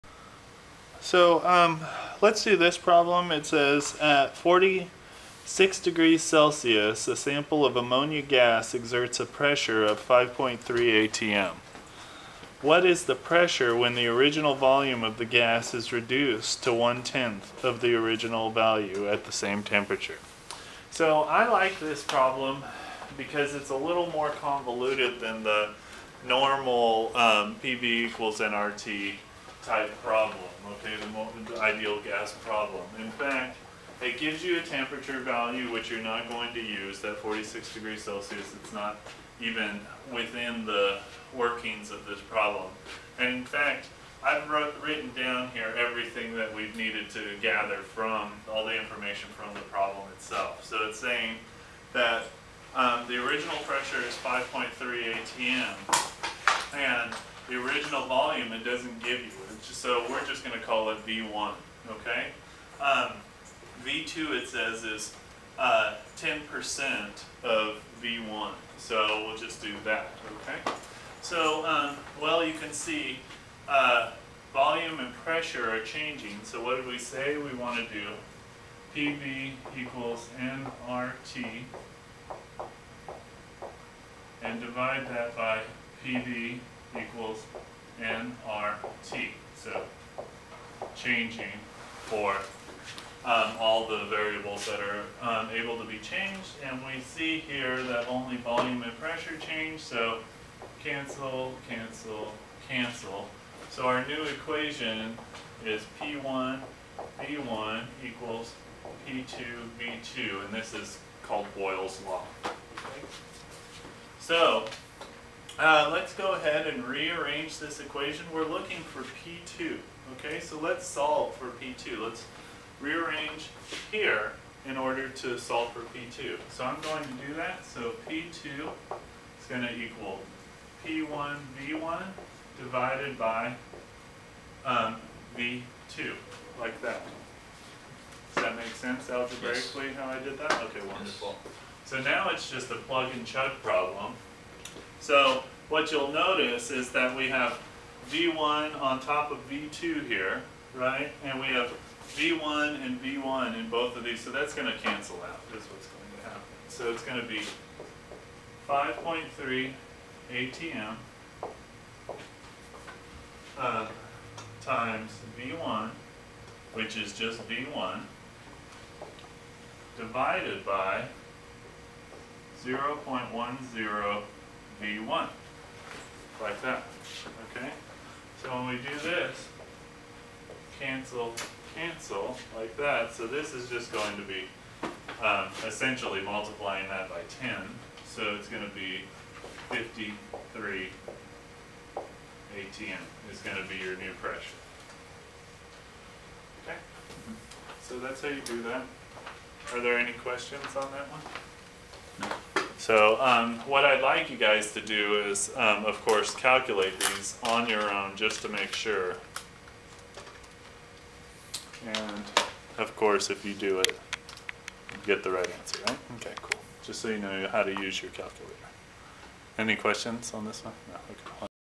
Boyle's Law 001
At 46 °C, a sample of ammonia gas exerts a pressure of 5.3 atm. What is the pressure when the volume of the gas is reduced to one-tenth of the original value at the same temperature?